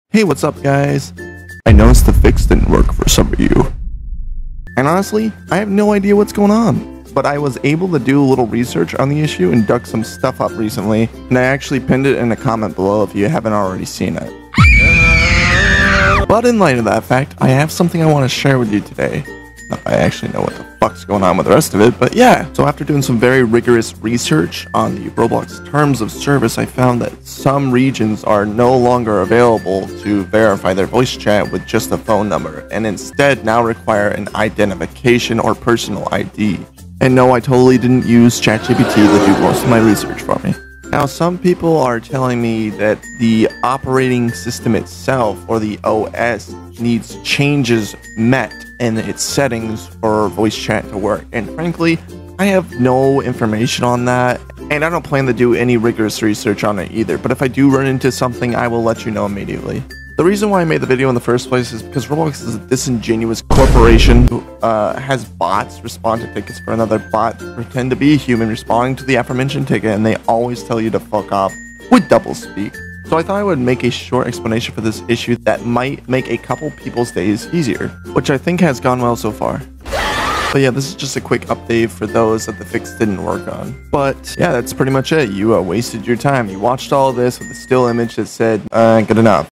How to Fix Roblox VOICE CHAT not showing up - UPDATE (Brief explanations & Rants)
A lot of this video is just rambling and brief explanation. Squander your time at your own risk..⚠️